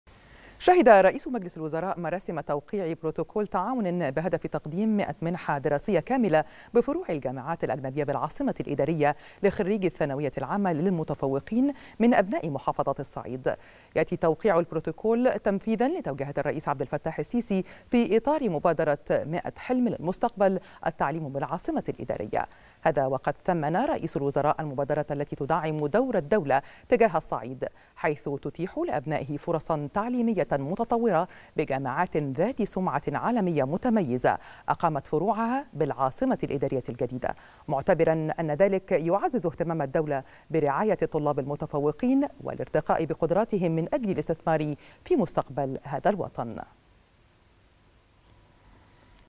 رئيس الوزراء يشهد توقيع بروتوكول لتقديم 100 منحة دراسية لمتفوقي الثانوية العامة بمحافظات الصعيد
نشرة السادسة | رئيس الوزراء يشهد توقيع بروتوكول لتقديم 100 منحة دراسية لمتفوقي الثانوية العامة بمحافظات الصعيد بالجامعات الأجنبية بالعاصمة الإدارية